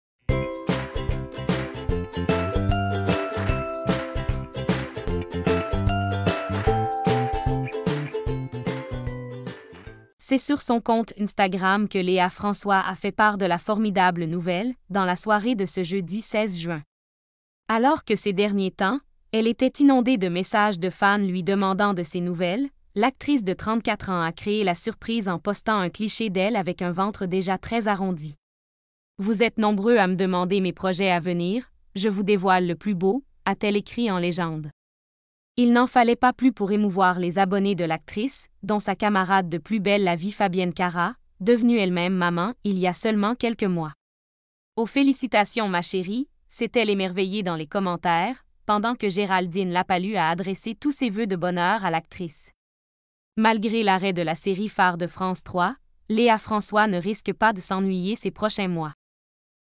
Léa François enceinte de son deuxième enfant : La star dévoile son ventre déjà très arrondi
Léa François a créé la surprise sur Instagram. Ce jeudi 16 juin, la star de la série Plus belle la vie a annoncé qu'elle attendait son deuxième enfant, tout en dévoilant un cliché de son ventre déjà très arrondi.